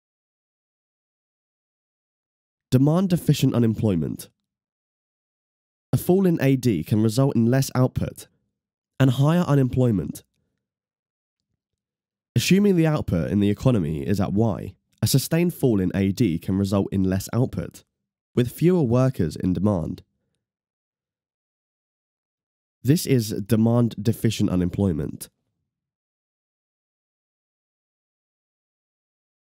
Demand deficient unemployment
Demand deficient unemployment occurs when aggregate demand is not sufficient enough to keep an economy near to 'full' employment.